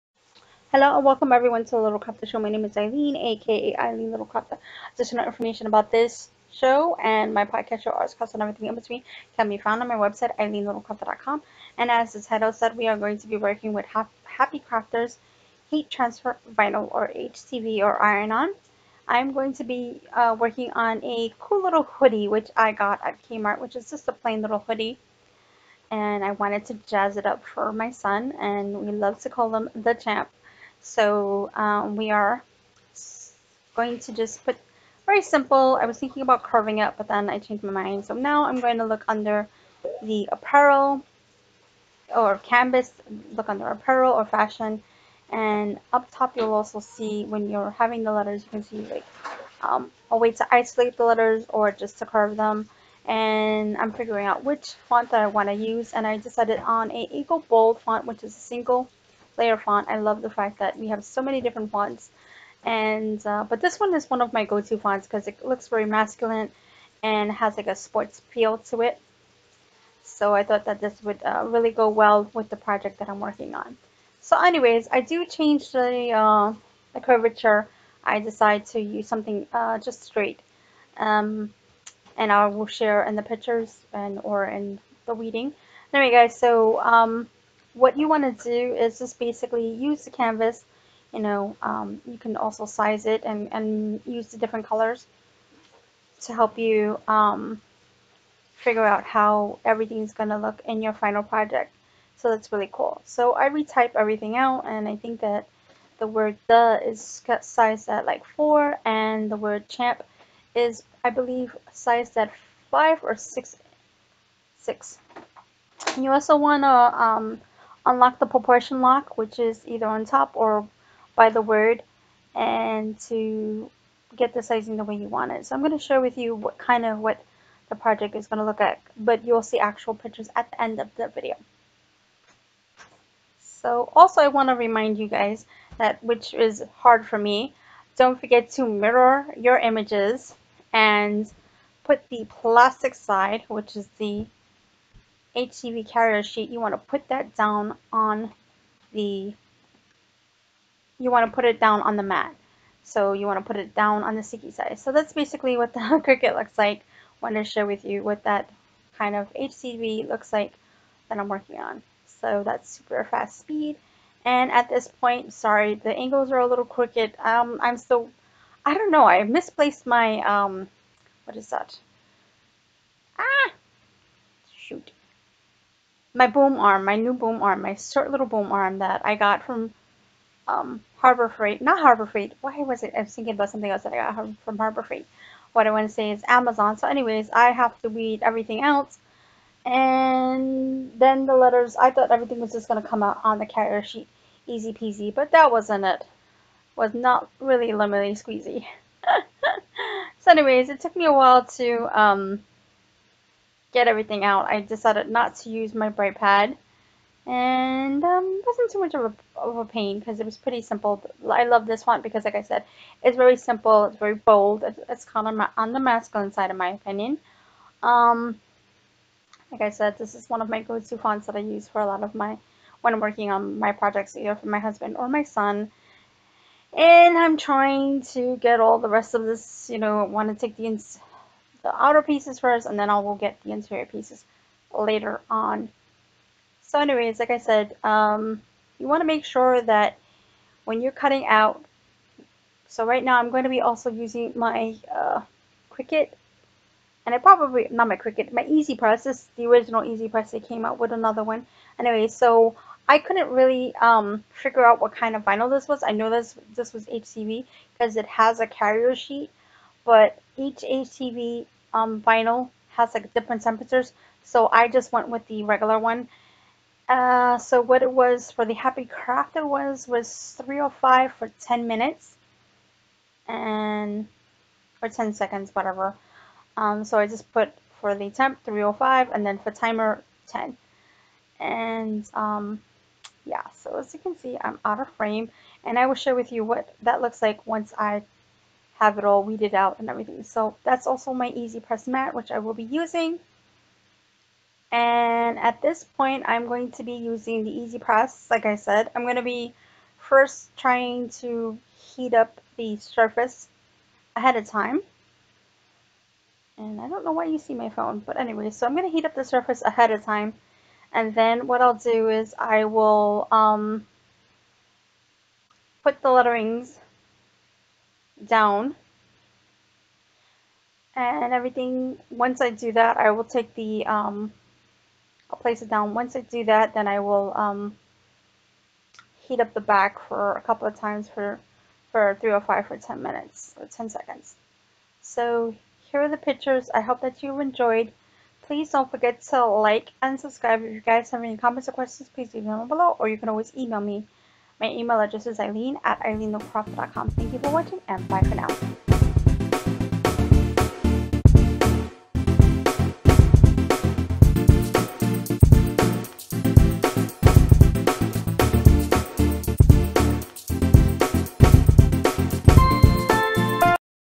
The little crafter show: Happy Crafters HTV on a Hoodie
I am so super excited to share a fun and quick project. Any kind of Heat transfer vinyl ( HTV) or iron vinyl will work, You need a blake item, which I am using a hoodie from Kmart. I am also using a Cricut maker, but any cutting machine will do, you need an iron and something to protect your work surf.  In the video, I am using Happy crafters HTV, Cricut maker, easy press and the easy press mat.
     I cut the letters out using the front called Eagle Bold, sized at 4x4 and 5x5. Once done I heated up my easy press to 305 & the timer to 10. After the letter where all ironed to the jacket, I then iron the inside of the project at the same temp, time and did that a couple of times. I then took off the heat transfer backing each of the letters to all done.

Zippered hoodie
Happy Crafters HTV vinyl
Cricut maker and design space
Easy press mat
Easy press